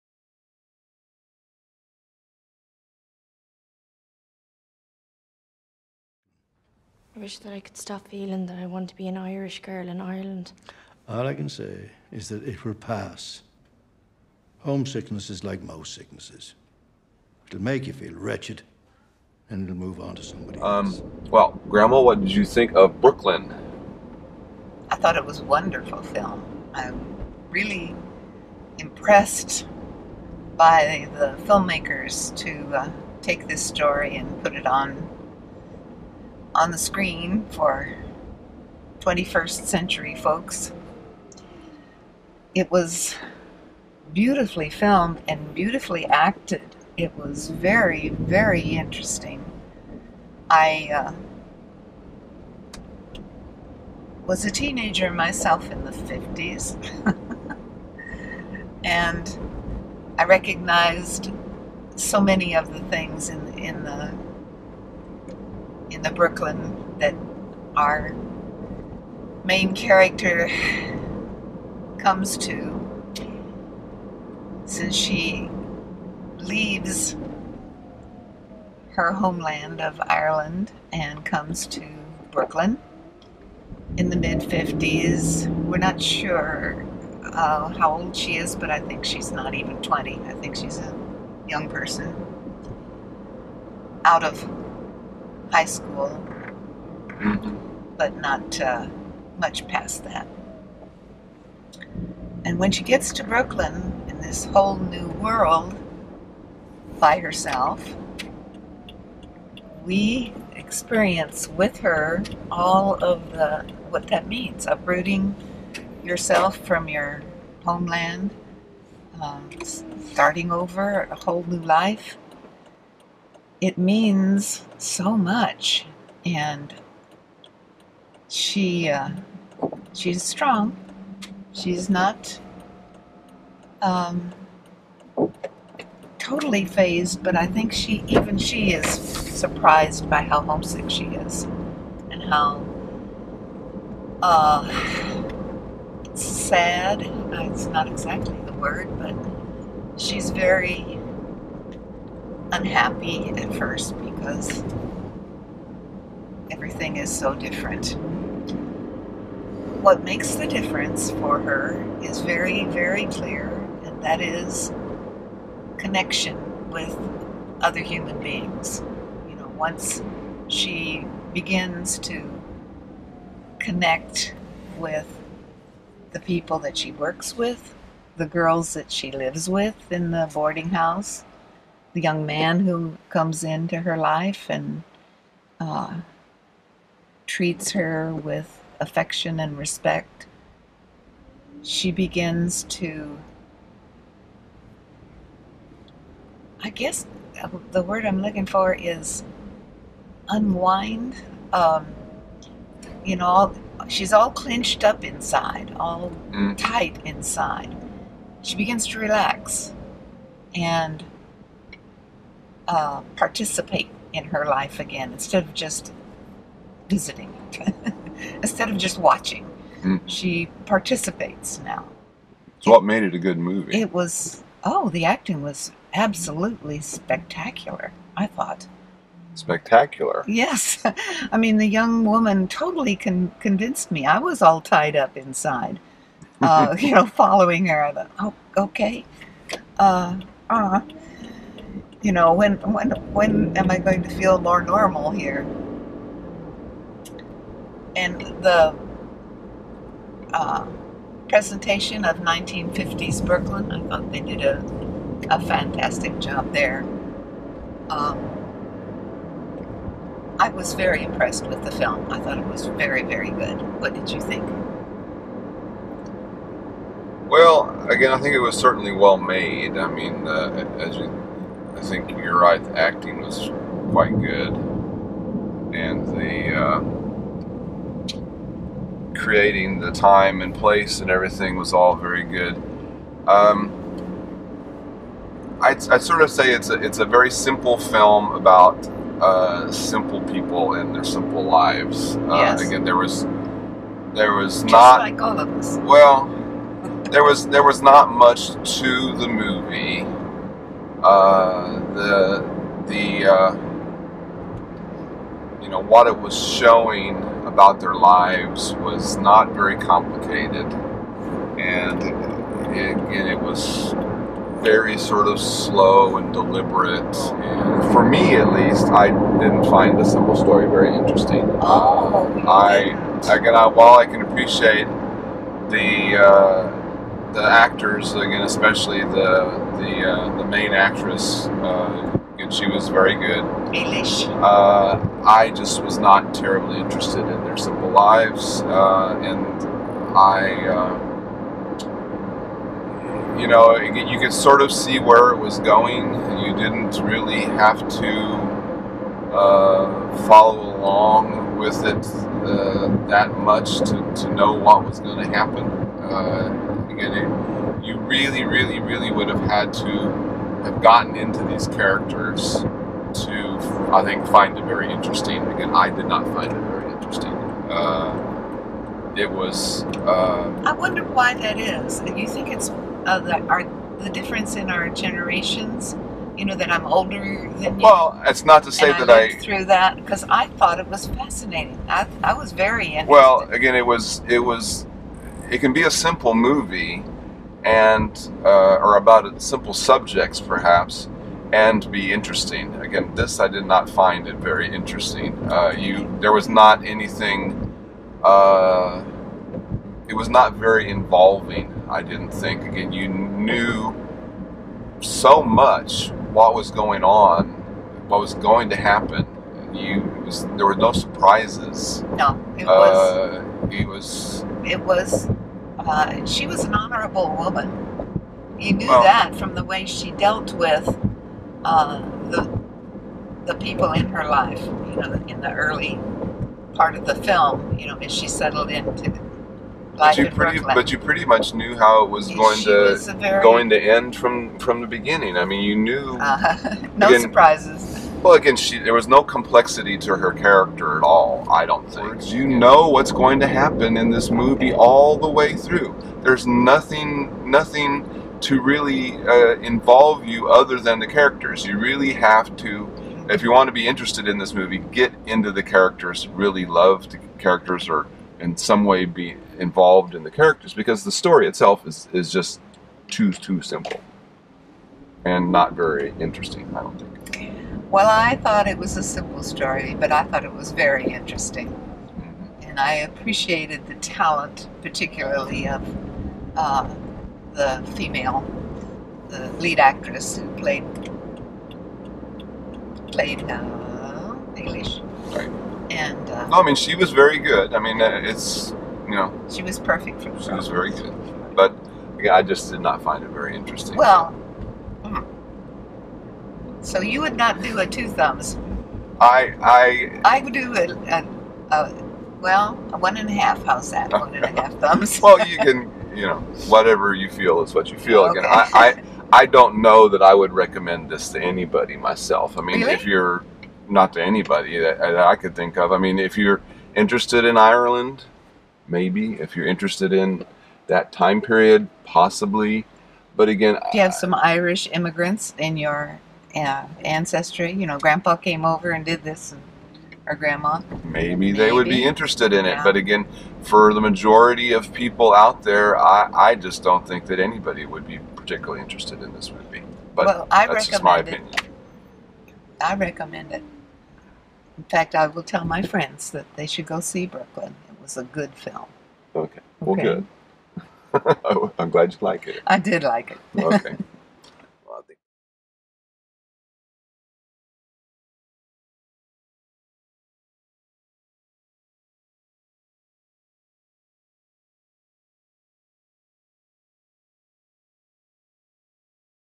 BROOKLYN review - Grandma goes to ther movies! (12-15-2015)
Rated PG-13, 1 hr. 51 min.
Tech notes: Recorded with a GoPro 3+
Much of it was filmed with the camera looking into the setting sun, so inside the car looks a little dark at times.